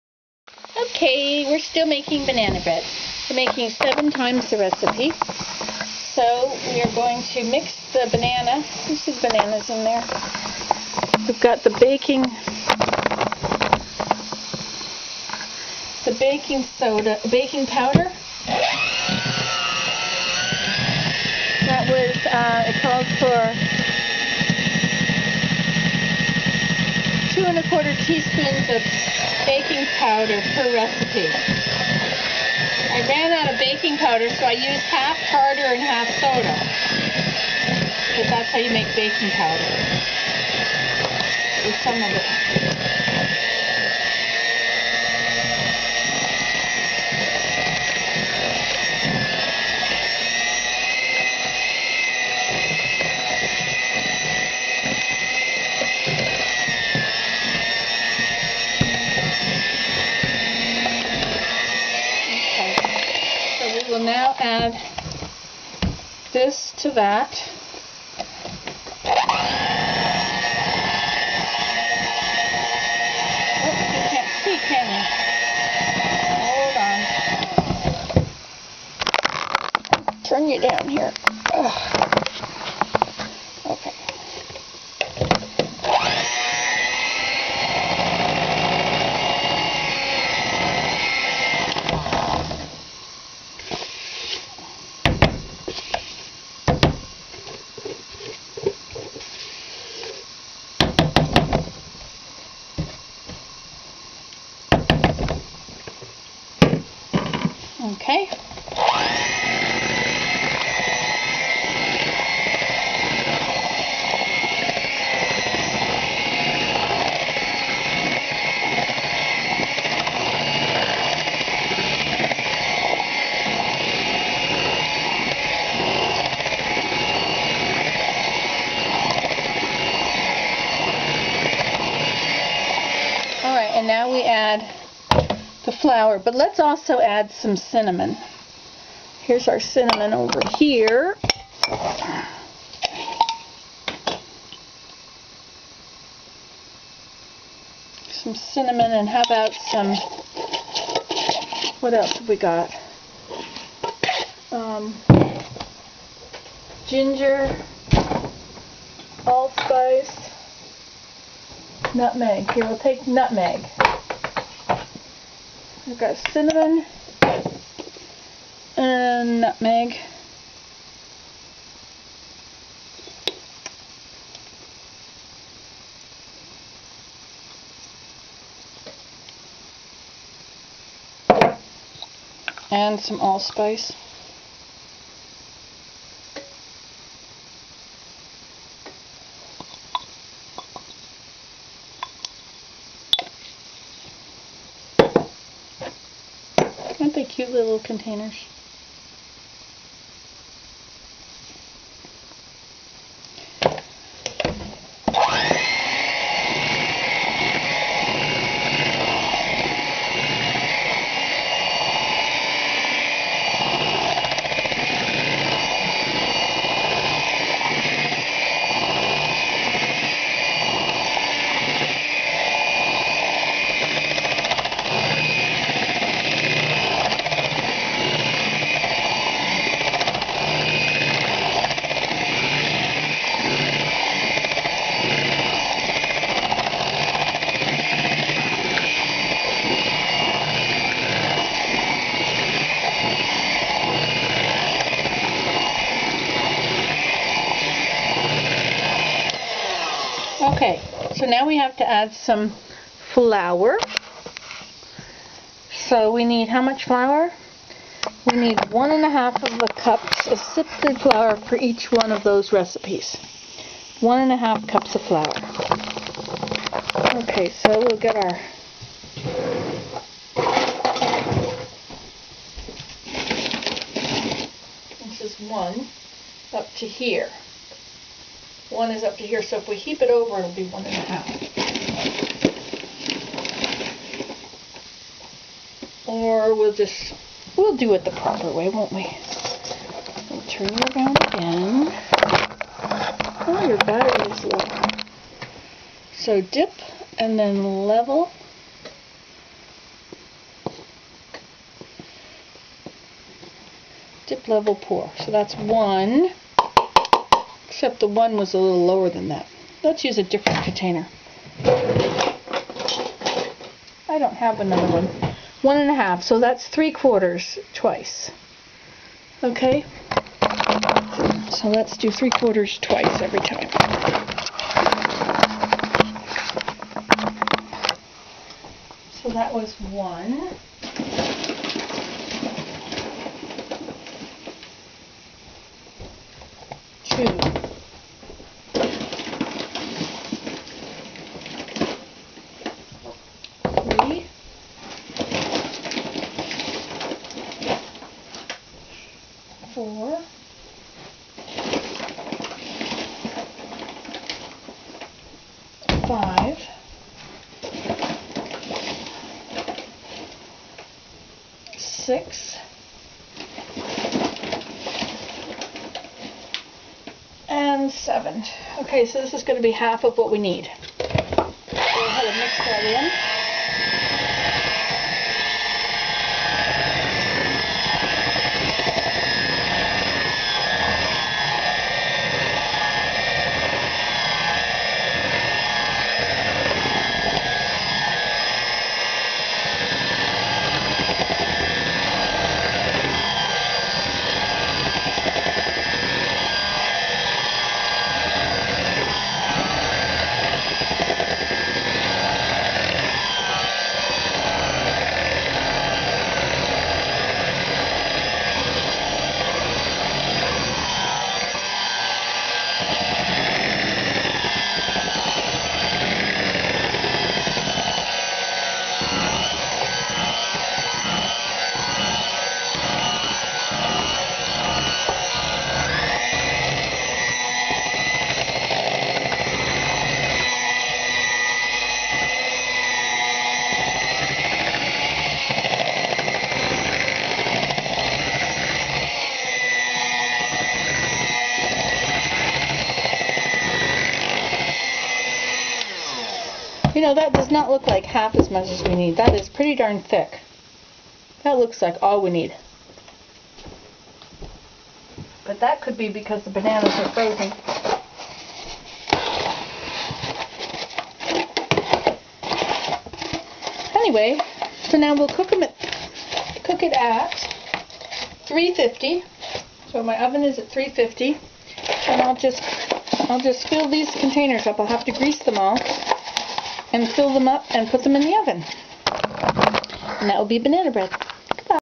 Nancy Today: How to make Banana bread 2
I`m making 7 times the recipe so we can eat one when it comes out of the oven, eat one later on in the day and freeze the rest!